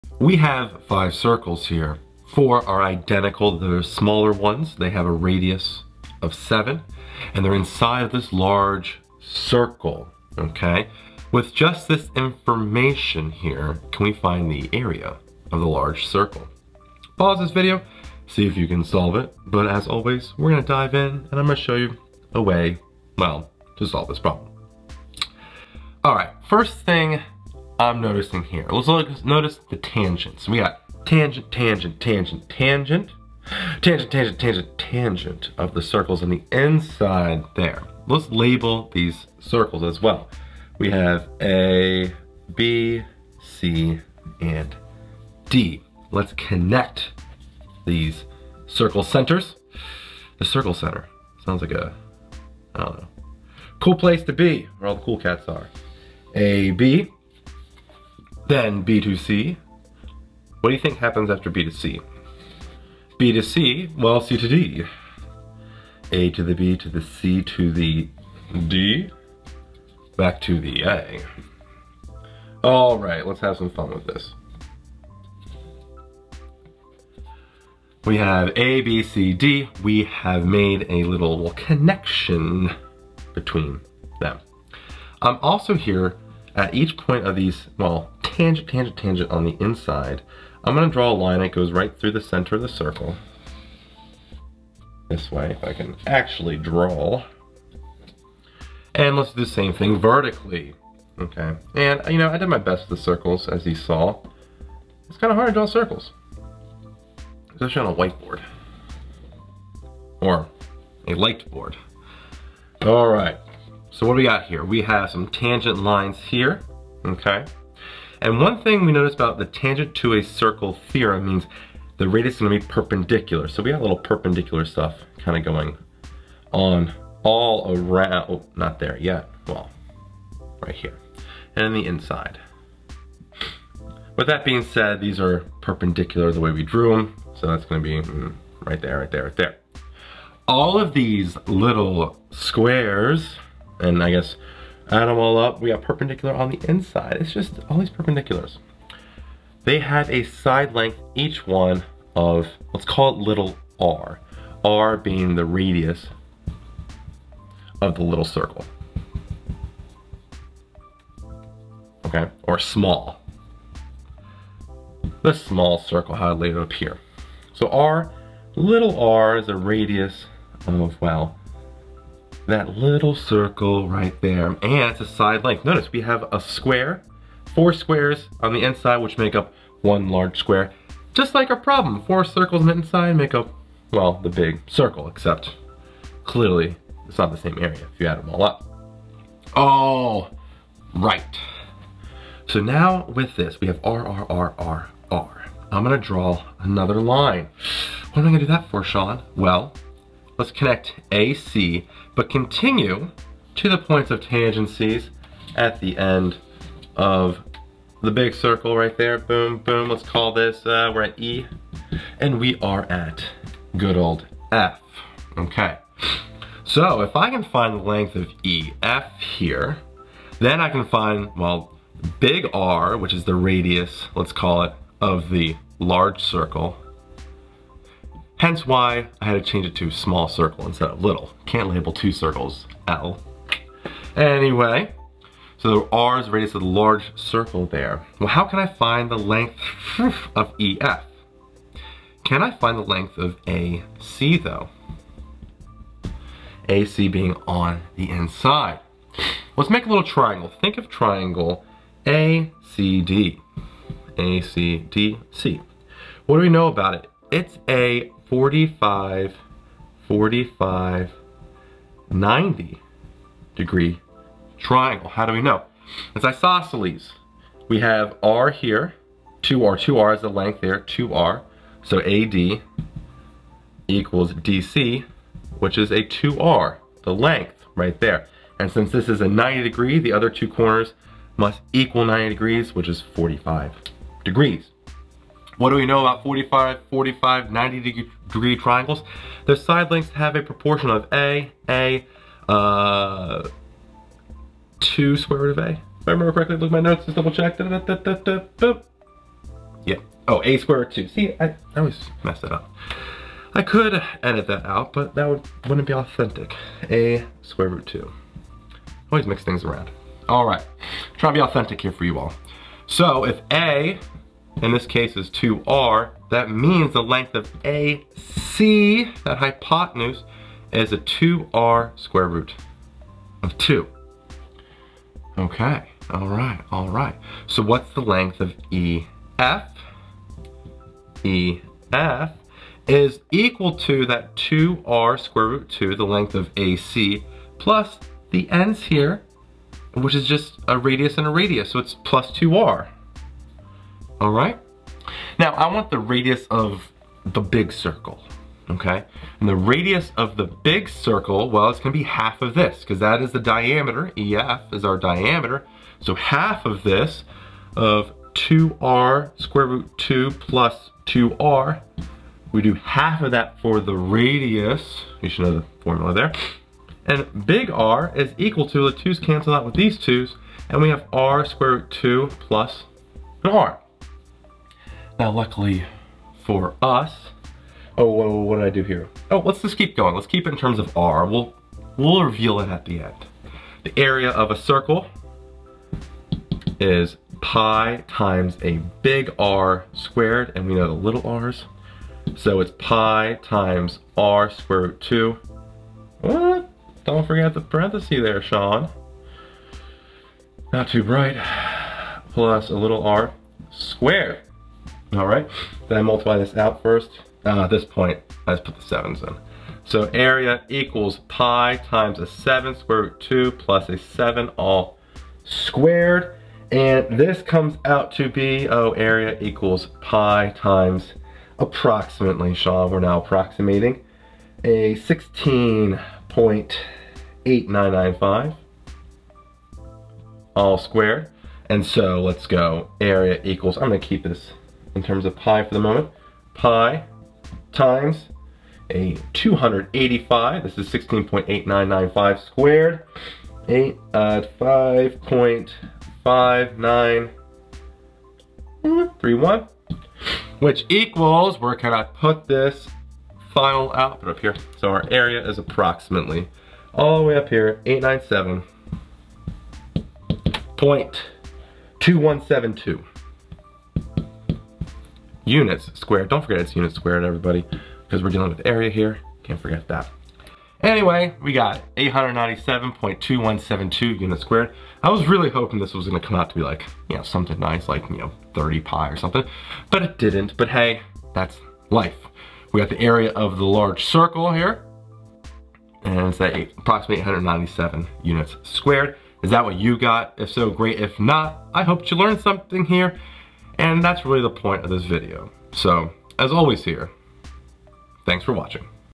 4 Small Circles, 1 Large Circle. What is the area of the Large Circle? | Minute Math
This hard geometry problem has 5 circles in it. 4 equal-sized small circles inside of 1 large circle. We know the small circles have a radius of 7. What is the area of the large circle? Use your knowledge of geometry to solve this problem!

Time Stamps:
00:00 - The Problem
00:25 - The Steps
06:57 - The Answer